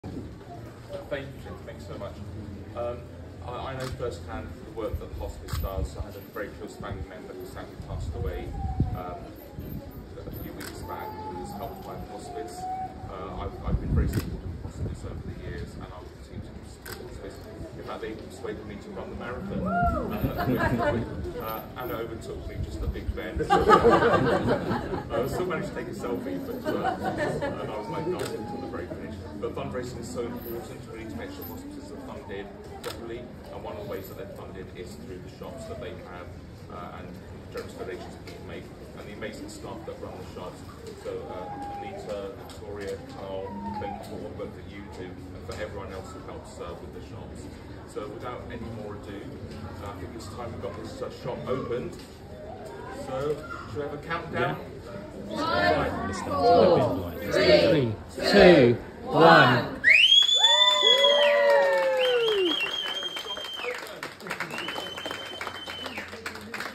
Local Parliamentary Candidate Opens Enfield Shop - North London Hospice
This morning we welcomed staff, volunteers, supporters, local residents and Bambos Charalambous, parliamentary candidate for Southgate and Wood Green, to open our brand new Enfield shop.

We would like to say a big thank you to Bambos for joining us for the opening. And thank you to Peter and the team from Landmark Builders for all their efforts in getting the shop ready for opening, and to our retail staff Catherine, Anita and volunteers, for all their hard work and dedication.

📍Palace Shopping Centre, 50 Cecil Rd, Enfield EN2 6SN